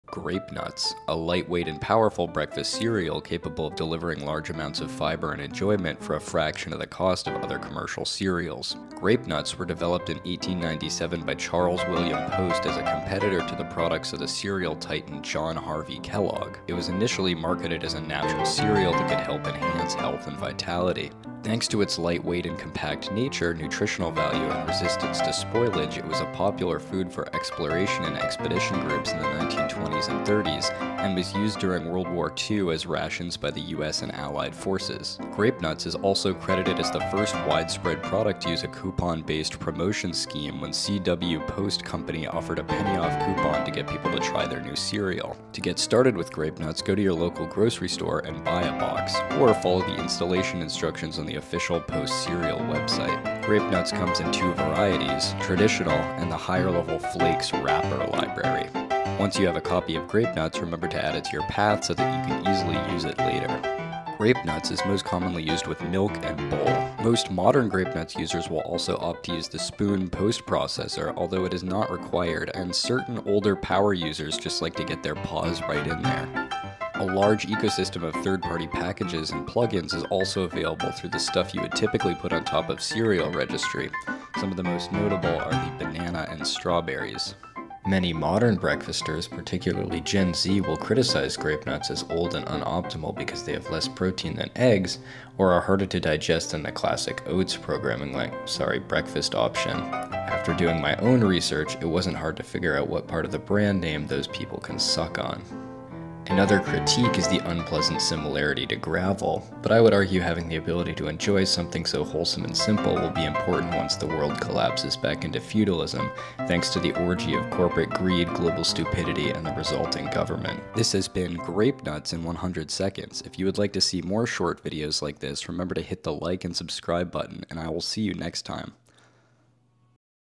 Grape nuts in 100 seconds
Grape nuts is a lightweight and powerful breakfast cereal, capable of insane compile times.

If you're reading a YouTube video description in your free time, you missed a funny Wester egg in the history of the channel.